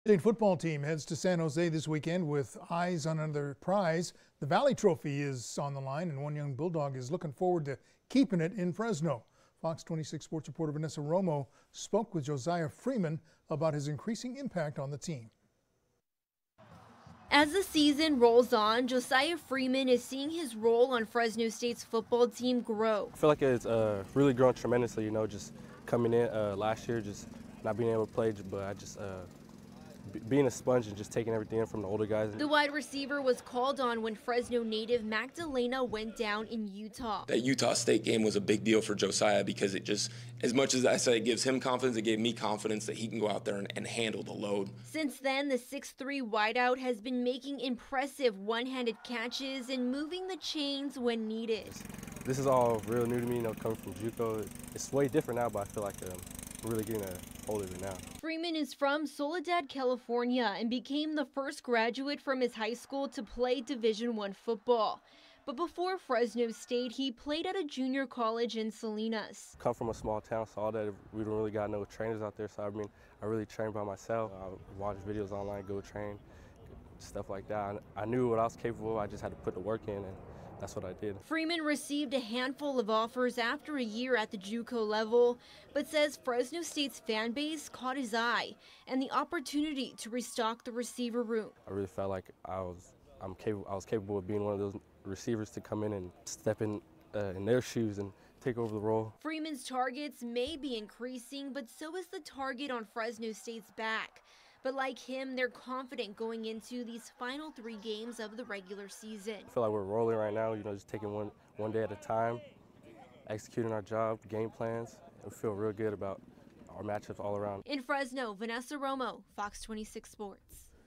SPORTS: Fresno State Bulldogs wide receiver Josiah Freeman
FOX26 sports reporter Vanessa Romo spoke with Fresno State wide receiver Josiah Freeman about his increasing impact on the team.